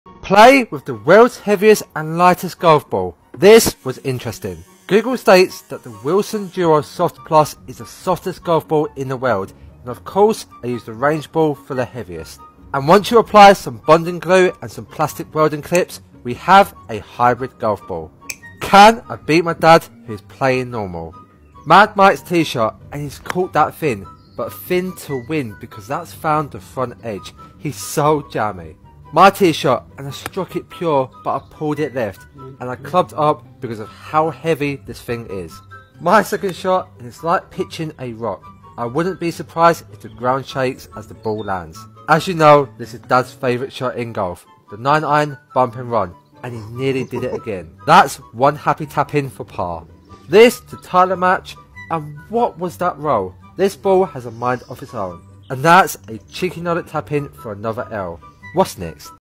HEAVIEST LIGHTEST HYBRID GOLF BALL - Father VS Son
Making a Hybrid Golf Ball - World's Heaviest Ball and Lightest Golf Ball
Can I Beat my Dad?! Or will Mad Mike go BACK-TO-BACK

WHAT'S NEXT??